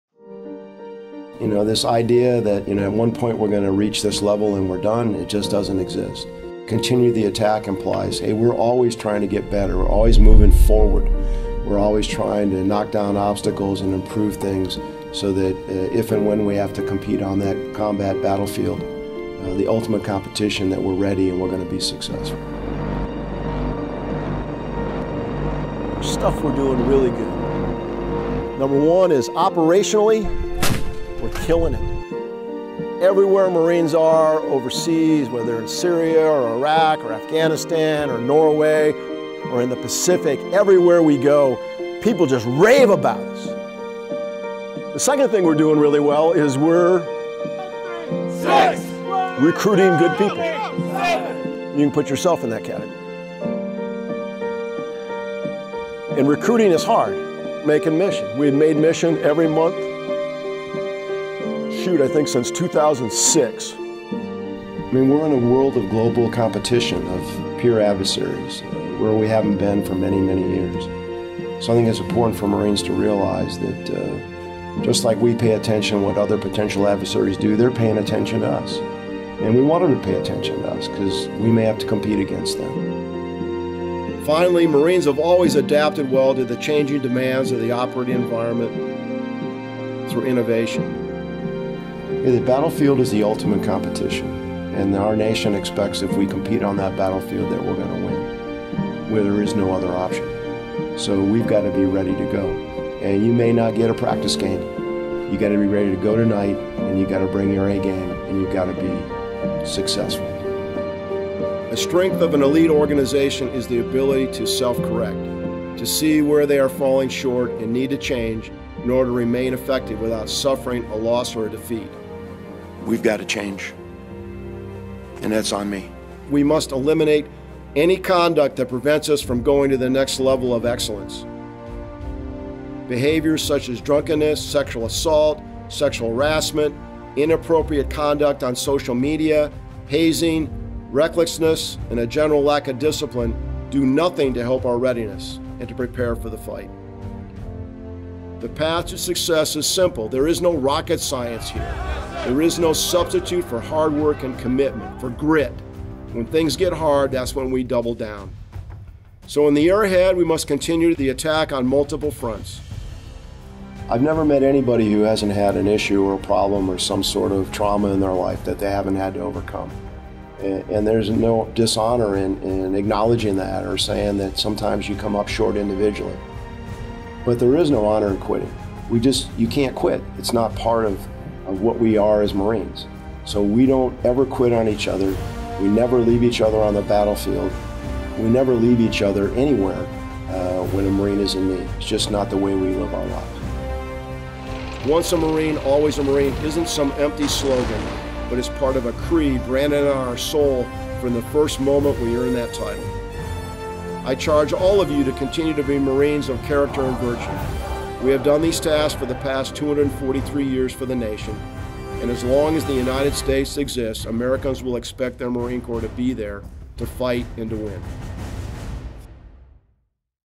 2019 CMC Message to the Force
Commandant of the Marine Corps, Gen. Robert B. Neller, speaks about his last Message to the Force, "Continue the Attack." This message talks about the strengths, challenges, and ways to move forward for the Marine Corps.